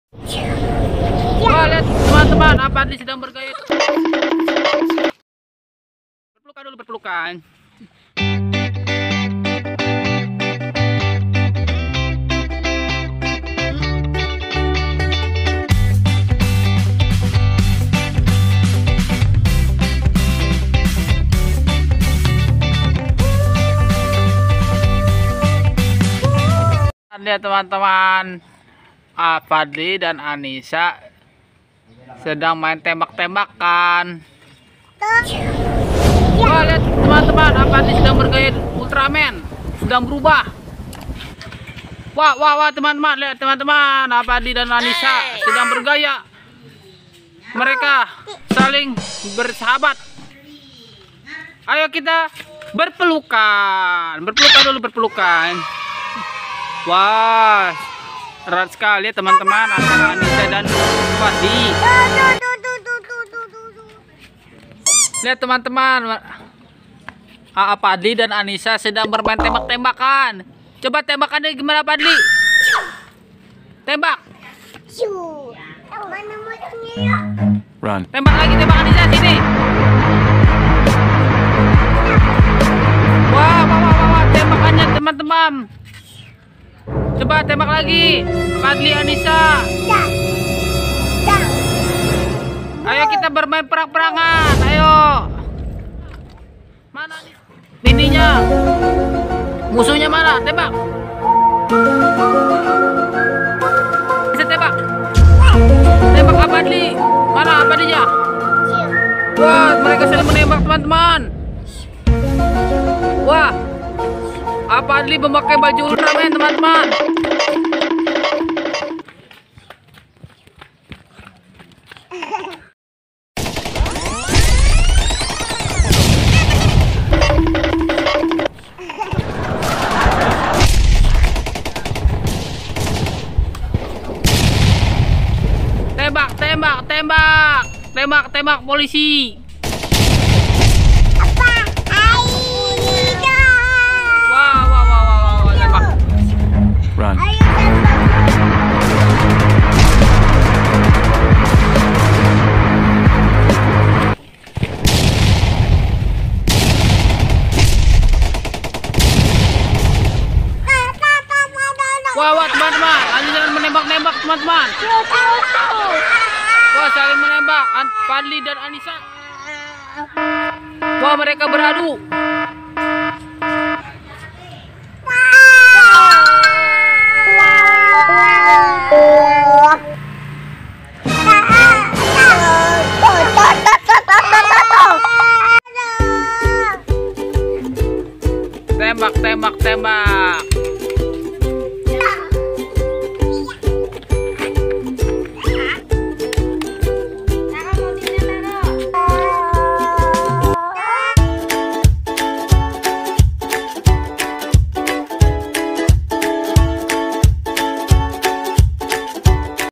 MELAWAN MUSUH ULTRAMAN | DRAMA MAIN tembak tembakan
Melawan musuh ultraman bermain tembak tembakan bermain bersama DeNisa Drama tembak menembak annisa dan fadli melawan musuh